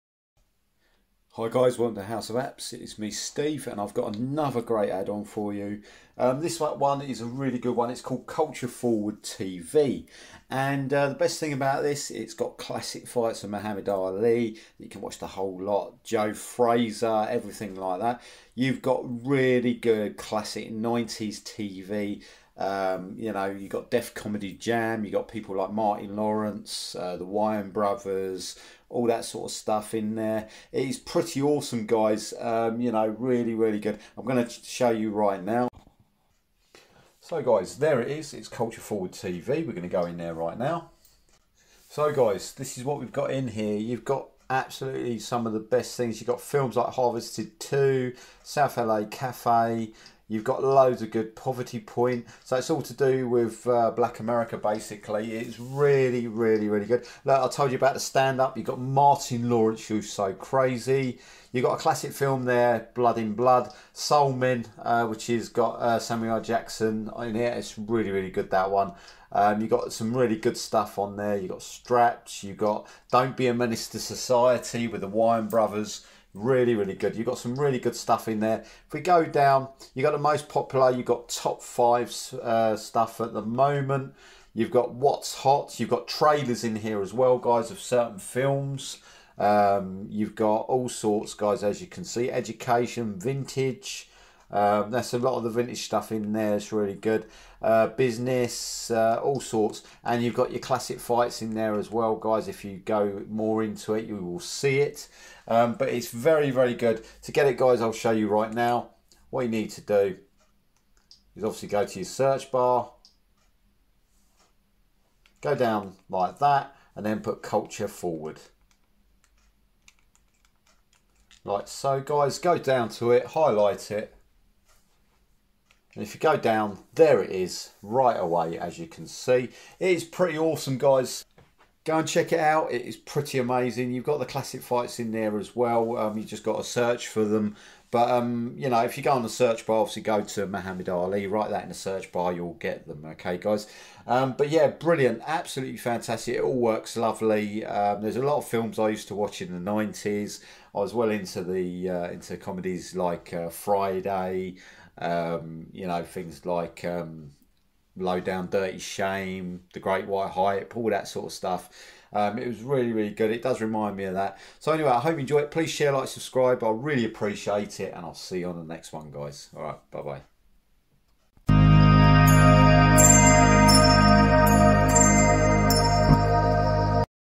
CULTURE FORWARD TV FOR FIRESTICK INCLUDING CLASSIC BOXING AND GREAT MOVIES
Great App with classic boxing, standup, documentaries, movies and all things urban.